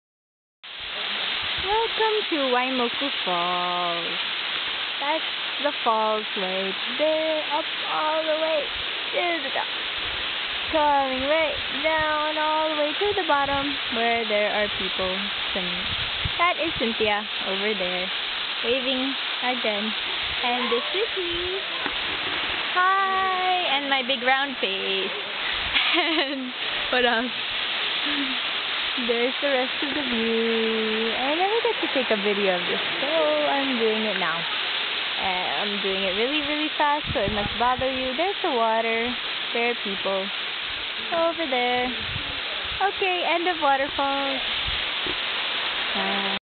Waimoku Falls, Maui
A shot of Maui's tallest waterfall.